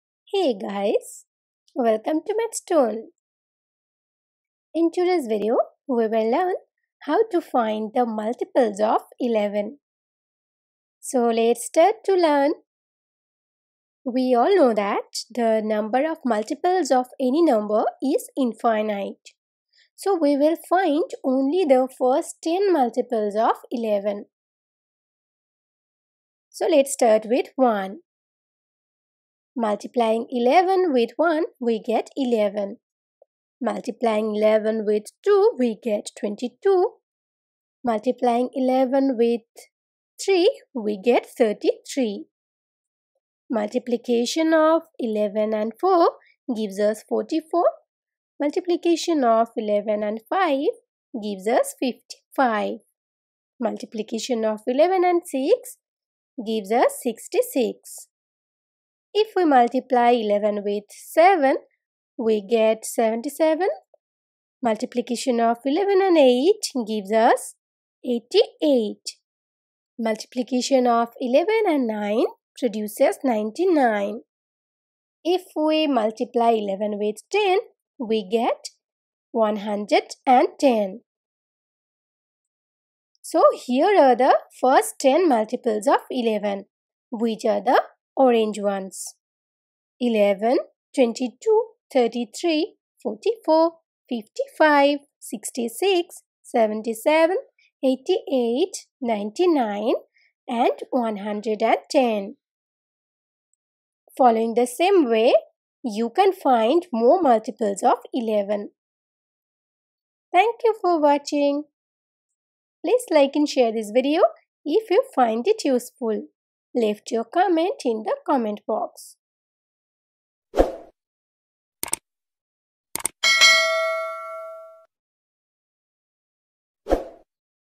Multiples of 11
In this video, we will find the multiples of 11 in a very easy way.
Topic: Multiple of 11 | Multiply by 11 | 11 times table | Multiplication table of 11

Question: What are the multiples of 11?
Answer: The multiples of 11 are 11, 22, 33, 44, 55, 66, 77, 88, 99, 110, etc.

We post videos daily on youtube